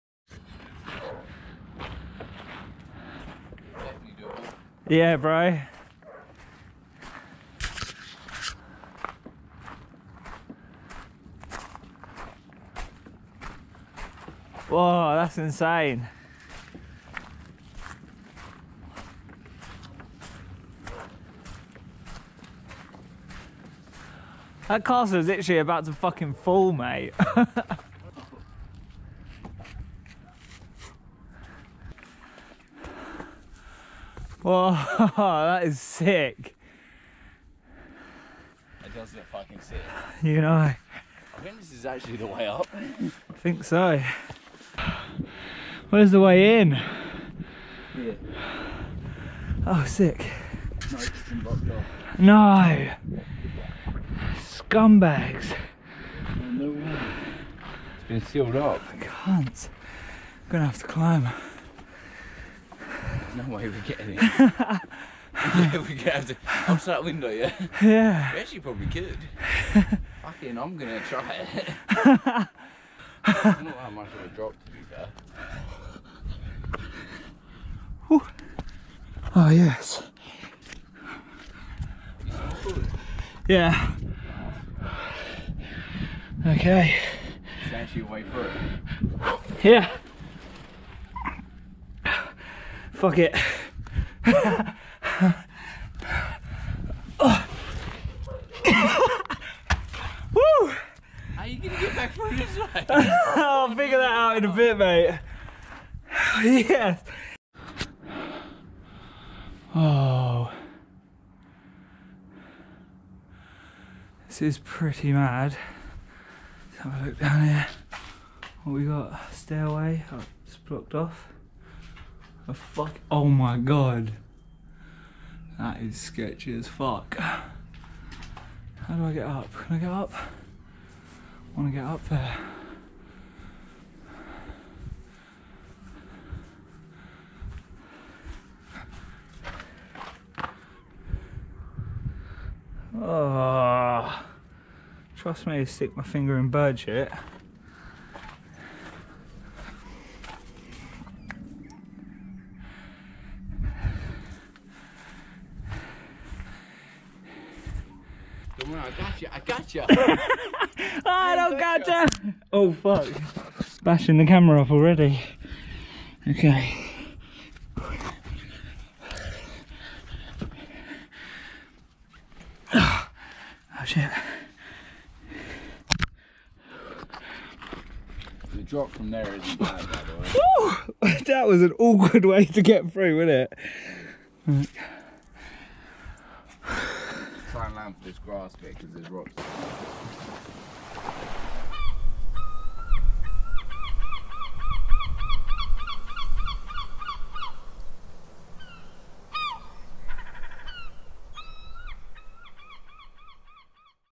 We Climbed in a Castle on the Edge of a Cliff | Greenan Castle
During our road trip to Scotland we visited and explored Greenan Castle

Filmed on Gopro Hero 7 Black & DJI Mavic Air